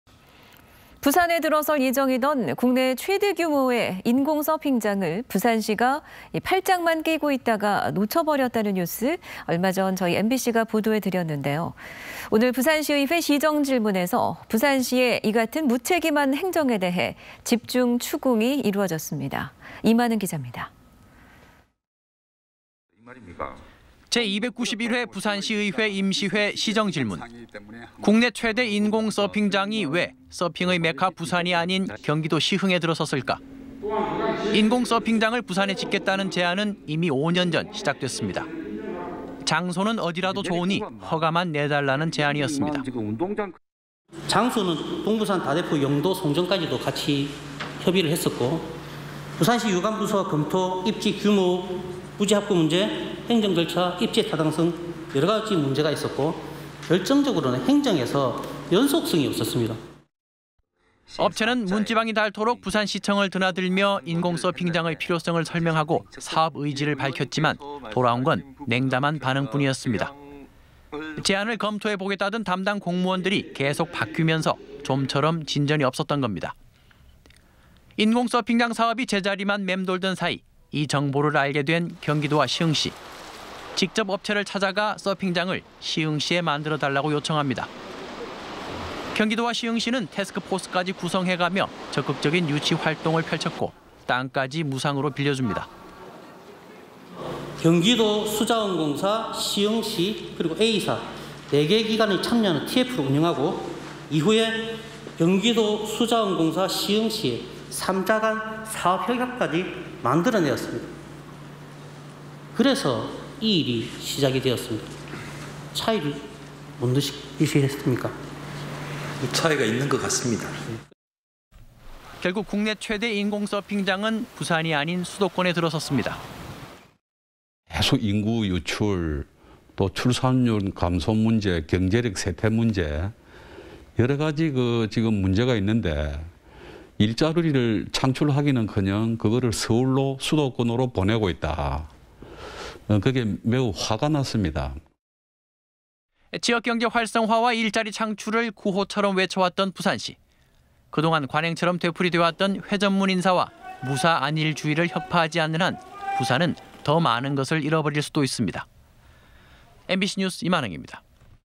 "국내 최대 인공서핑장 시흥으로 간 이유는?" (2020-10-21/MBC 뉴스데스크 부산)
▶ 부산의 한 업체가 부산시에 인공서핑장 제안했지만 부산시는 5년동안 검토만하다 인공서핑장을 경기도 시흥시에 빼앗기고 말았습니다. 시의회 시정질문에서 그 경위를 집중 추궁했는데, 근본적인 문제는 부산 공무원들의 무사 안일주의와 회전문 인사였음이 드러났습니다.